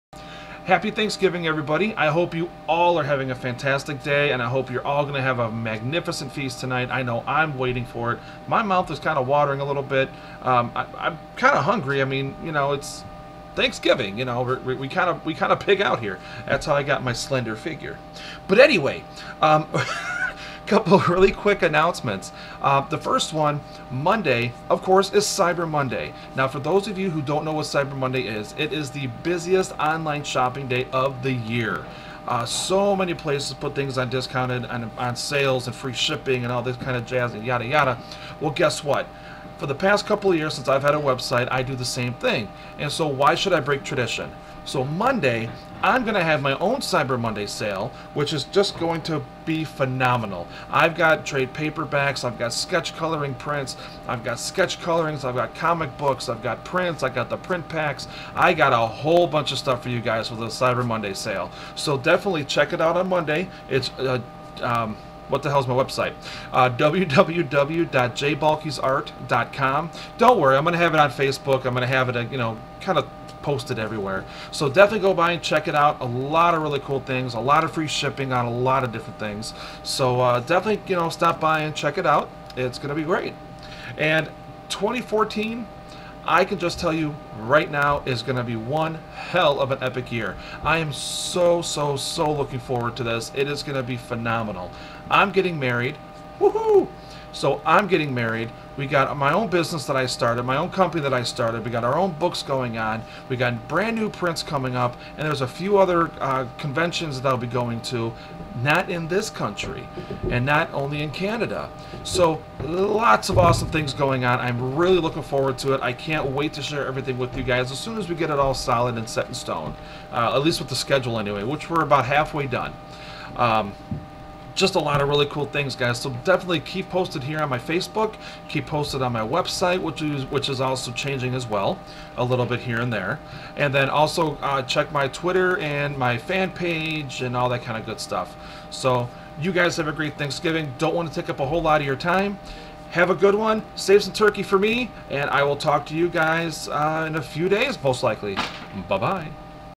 HAPPY THANKSGIVING and CYBER MONDAY
Happy Thanksgiving to EVERYONE..and Cyber Monday, I'm having a HUGE sale on a LOT of items including:
- Trade Paper Backs
- Sketch Coloring Prints
- Prints
- Comic Books

for what I'll have starting on Cyber Monday next week..and going until Friday of next week!